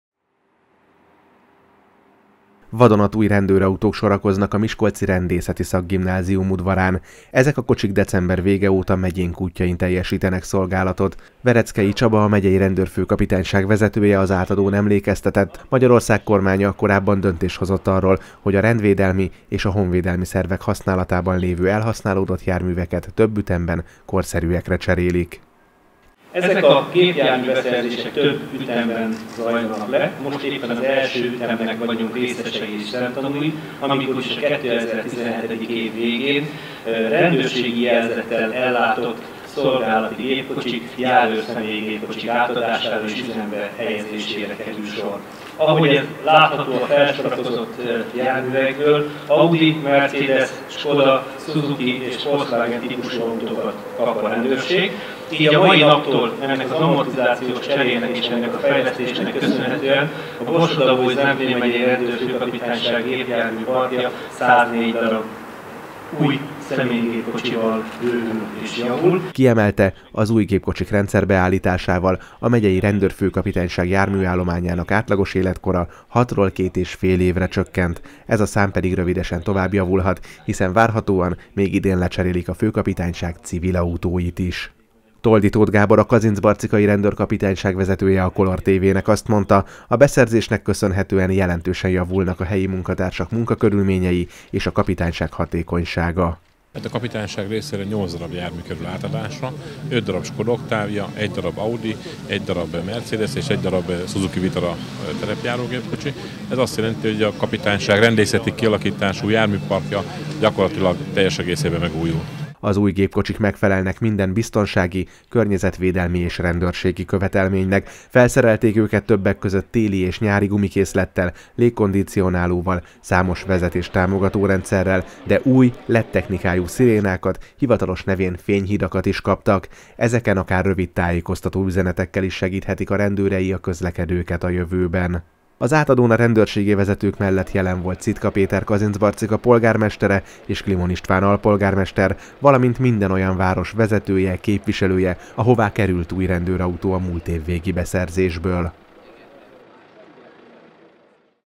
KOLORTV - Új rendőrkocsik
Több mint száz új rendőrautóval gazdagodott a Borsod-Abaúj-Zemplén Megyei Rendőr-főkapitányság állománya. Az új járművek közül több Kazincbarcikán lát el szolgálatot.